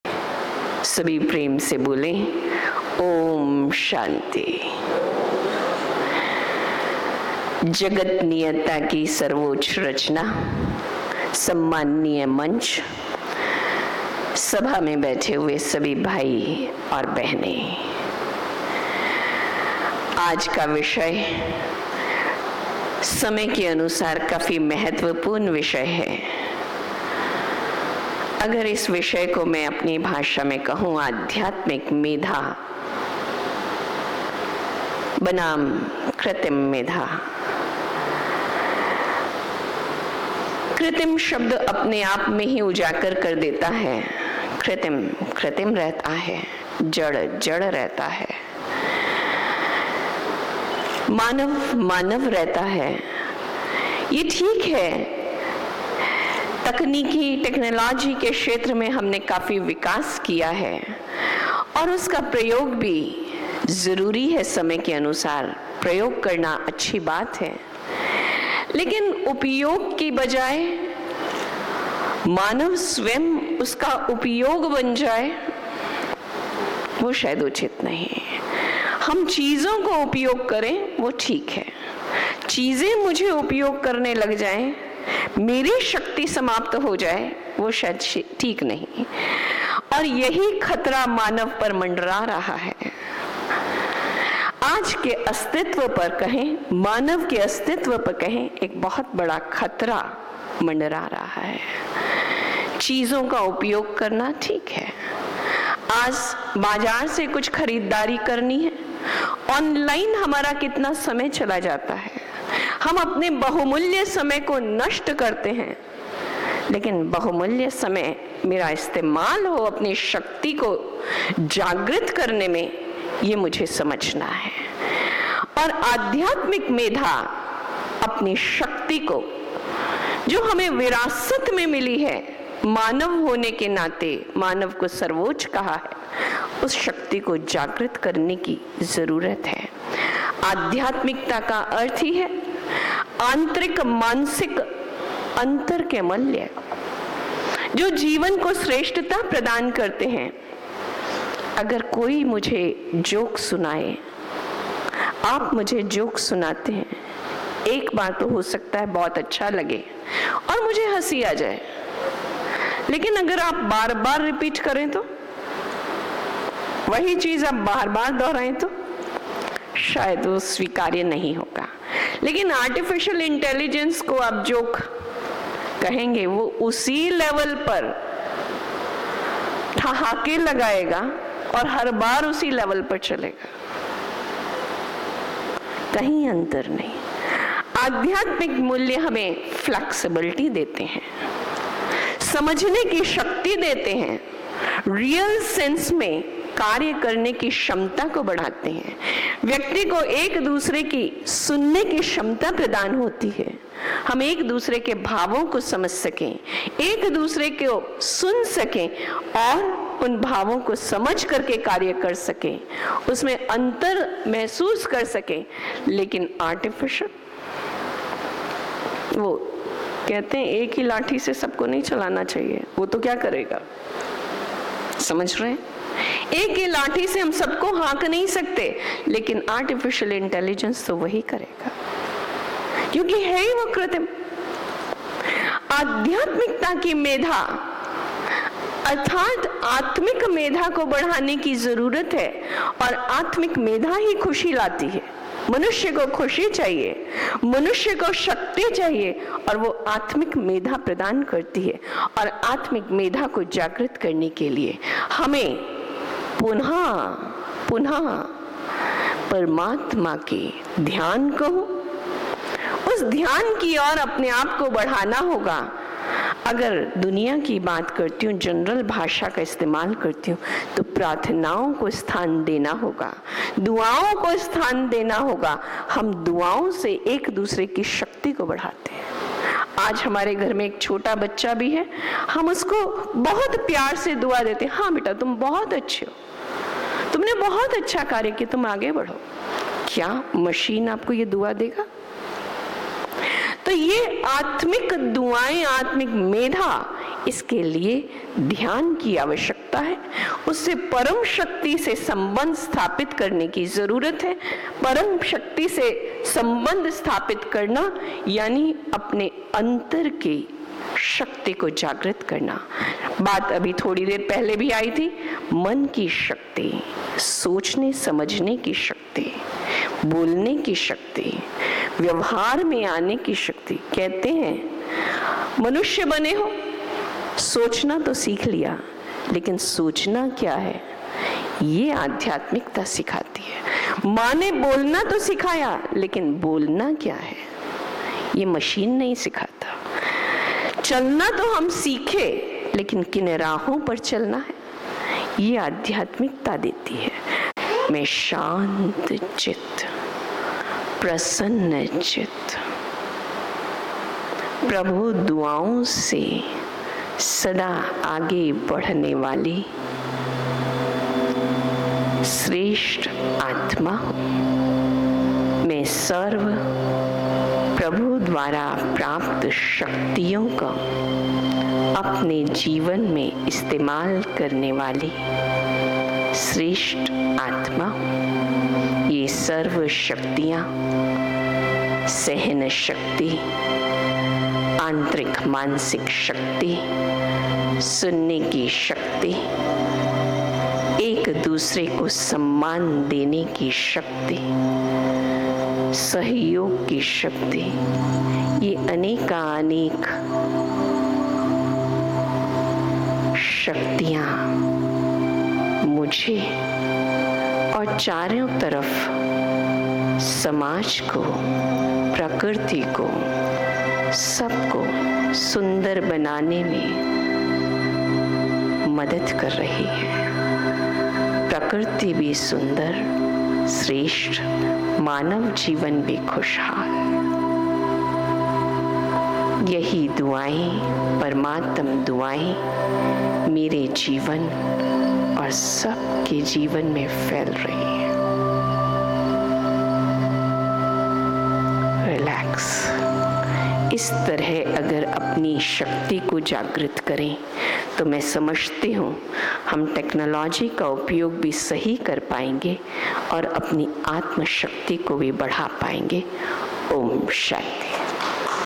National Media Conference | Plenary2 | B K Chandrakala ZCO Media Wing Jaipur|BrahmaKumaris | Mt.Abu
National Media Conference & Retreat [ 23-27 May 2024 ]

“Vision and Values for a New Social Order- Role of Media”
नई सामाजिक व्यवस्था के लिए दृष्टि और मूल्य- मीडिया की भूमिका;

⚜️Plenary Session-II -25/05/2024, 4.00 pm -Topic : Spiritual Intelligence vs. Artificial Intelligence (आध्यात्मिक बुद्धिमत्ता बनाम कृत्रिम बुद्धिमत्ता)

📌Speaker : B K  Chandrakala Zonal Coordinator Media Wing Jaipur

 * Venue : Academy for a Better World, Brahma Kumaris
Gyan Sarovar, Mt. Abu (Raj.)

Organised & Hosted by:  Media Wing, Prajapita Brahma Kumaris Ishwariya Vishwa Vidyalaya
World Headquarters: Mount Abu (Raj.)